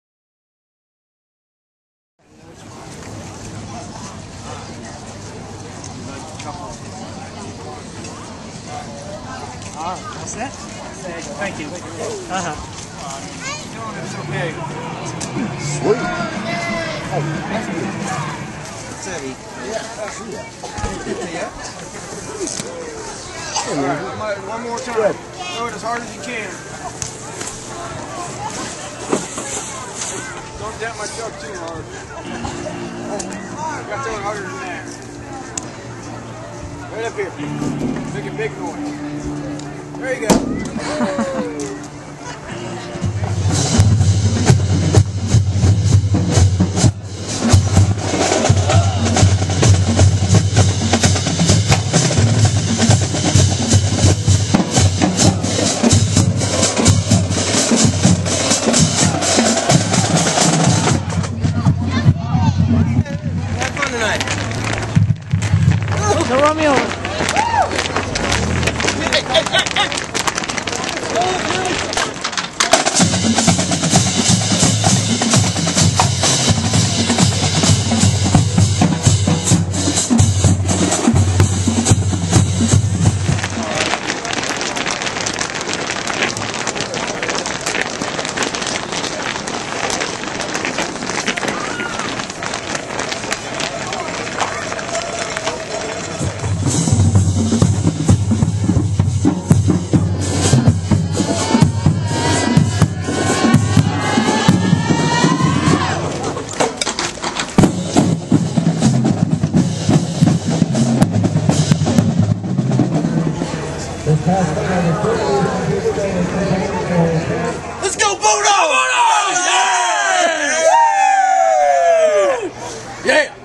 The Basement Doctor Food Drive Elida vs Wapak Football
this was our last football game food bank drive at Elida highschool vs Wapakoneta